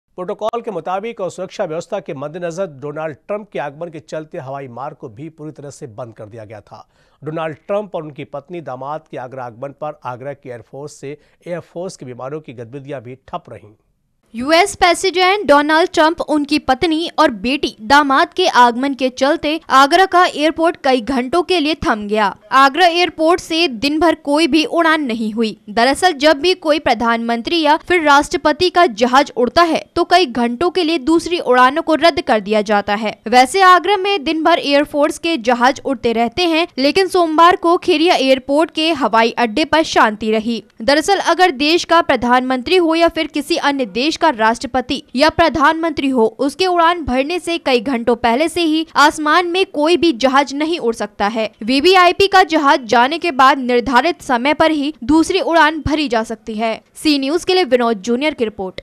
सोमवार को आगरा का एयपोर्ट रहा शांत
डोनाल्ड ट्रंप के दौरे के चलते नहीं हुई कोई उड़ान:
प्रोटोकाॅल के मुताबिक और सुरक्षा व्यवस्था के मददेनजर डोनाल्ड ट्रंप के आगमन के चलते हवाई मार्ग को भी पूरी तरह से बंद कर दिया गया था। डोनाल्ड ट्रंप और उनकी पत्नी दामाद के आगरा आगमन पर आगरा के एयरफोर्स से एयरफोर्स के विमानों की गतिविधियां ठप रहीं।

देश के प्रधानमंत्री, राष्ट्राध्यक्ष के लिए हैं ये नियम:
यूएस राष्ट्रपति डोनाल्ड ट्रंप उनकी पत्नी और बेटी दामाद के आगमन के चलते आगरा का एयरपोर्ट कई घंटों के लिए थम गया। आगरा एयरपोर्ट से दिनभर कोई भी उड़ान नहीं हुई । दरअसल जब भी कोई प्रधान मंत्री या फिर राष्ट्रपति का जहाज उड़ता है तो कई घंटों के लिए दूसरी उड़ानों को रदद कर दिया जाता है। वैसे आगरा में दिन भर एयरफोर्स के जहाज उड़ते रहते हैं लेकिन सोमवार को खेरिया एयरपोर्ट का हवाई अडडे पर शांति रही। दरअसल अगर देश का प्रधानमंत्री हो या फिर किसी अन्य देश का राष्ट्रपति या प्रधानमंत्री हो उसके उड़ान भरने से कई घंटे पहले से ही आसमान में कोई भी जहाज नहीं उड़ सकता है। वीवीआईपी का जहाज जाने के बाद निर्धारित समय पर ही दूसरी उड़ान भरी जा सकती है।